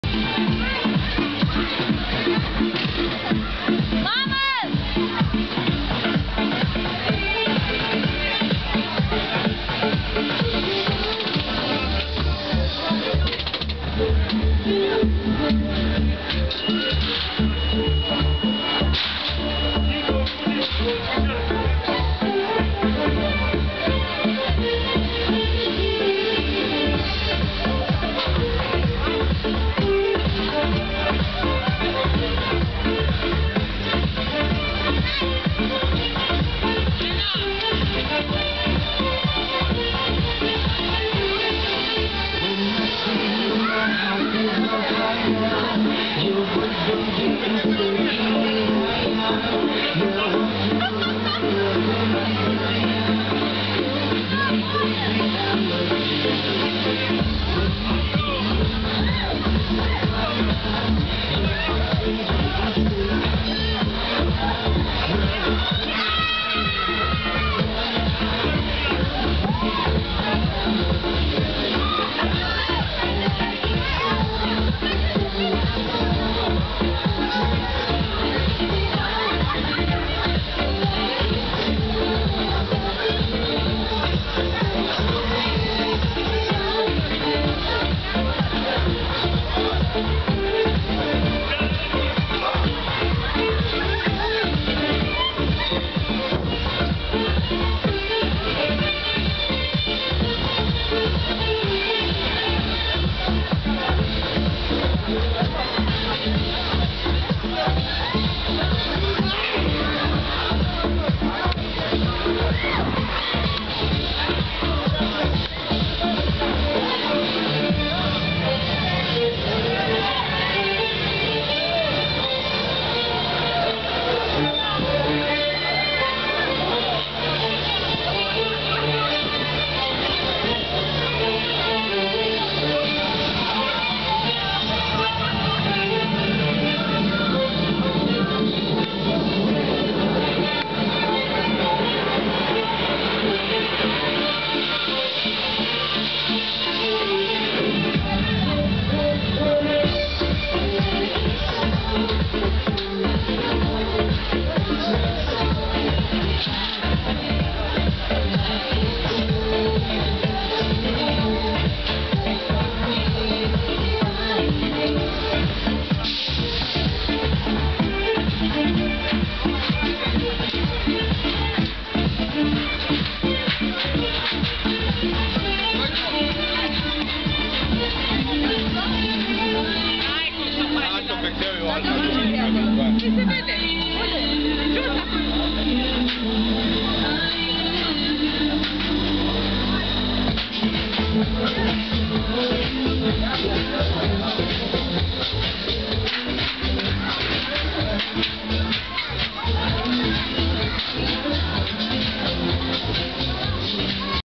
Xtreme Spinner- May 22, 2011- very close during the ride.
Very  closeduring the operation  of the  Xtreme Spinner- the evening of May 22, 2011 at 20:42.